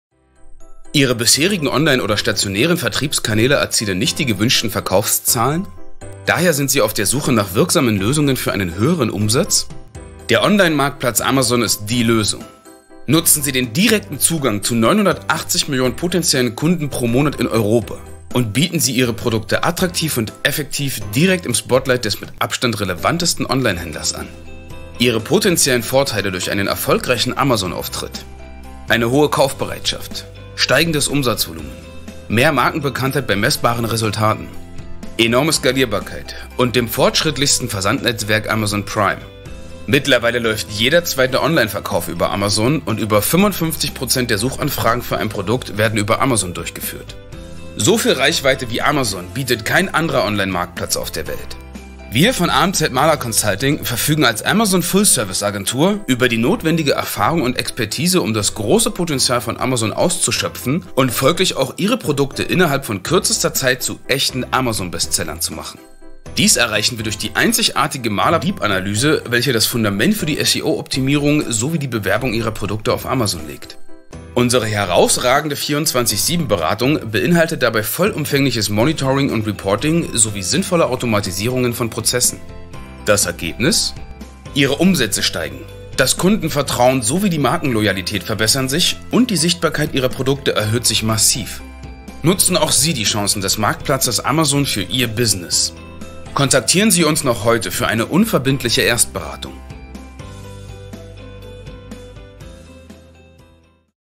Amzmala | Animiertes Erklärvideo von UPJorouge
This is an example of how an explainer video can convey the relevance of your business to your consumers in a fun and exciting way.

Do you need a similar animation for your company, product, and service?